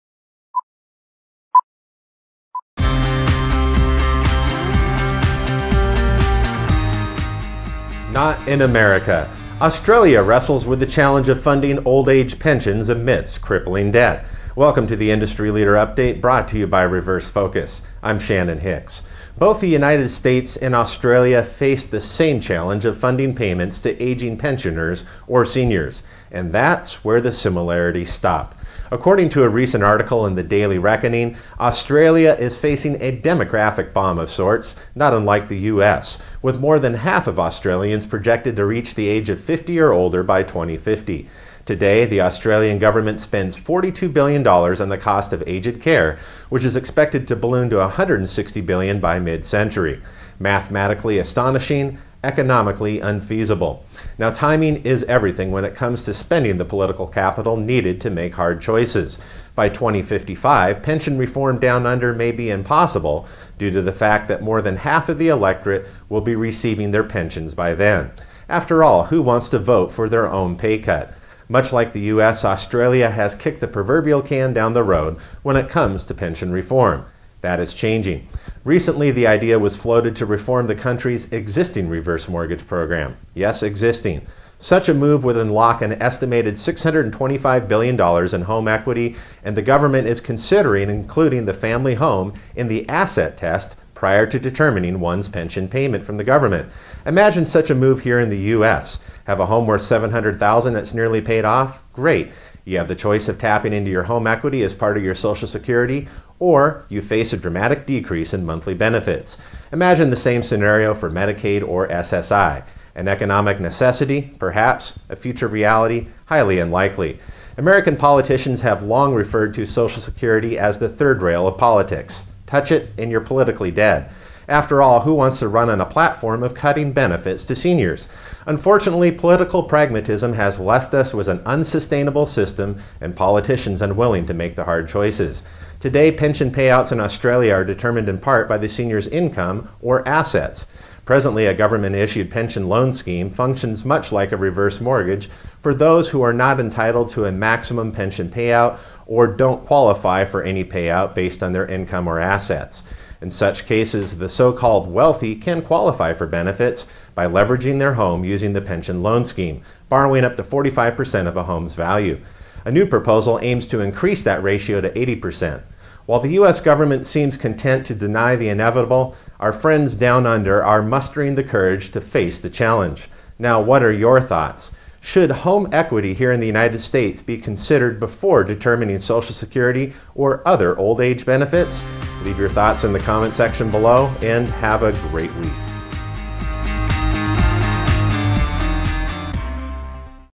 Reverse Mortgage News: Not in America
Australia wrestles with the challenge of funding old age pensions amidst crippling debt. Both the United States and Australia face the same challenge of funding payments to aging pensioners or seniors, and that’s where the similarities stop...